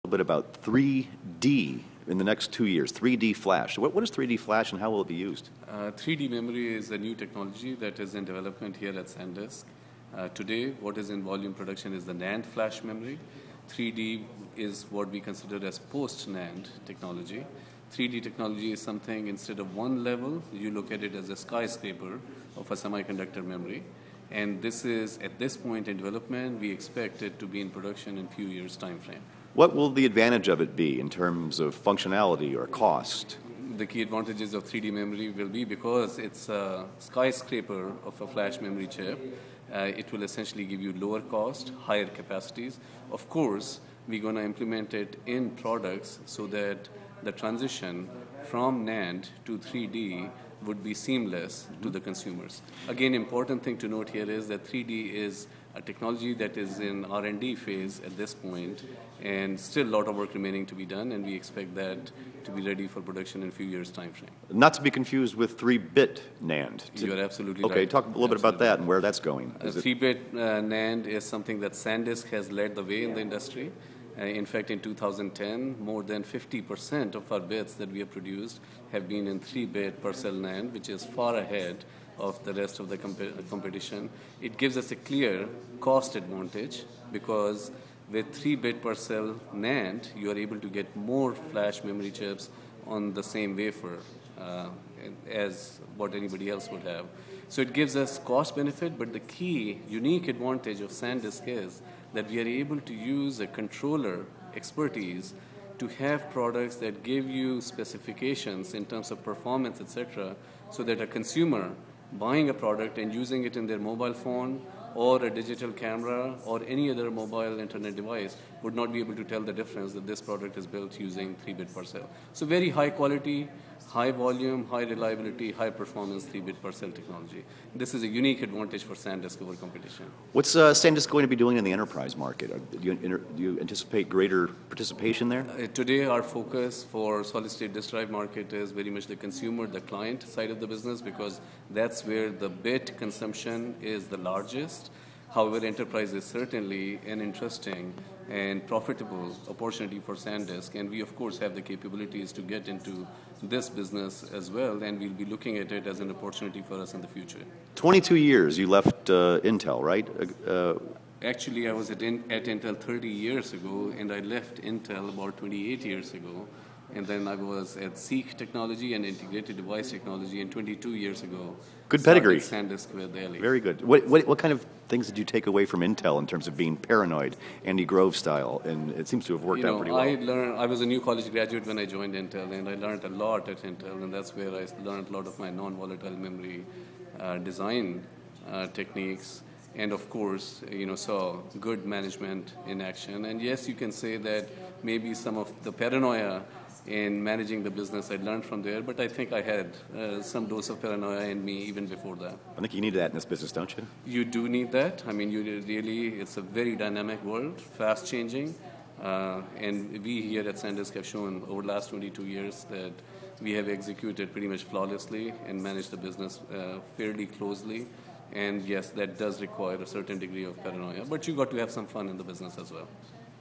Sanjay Mehrotra
SanDisk COO talks memory business and where it's going.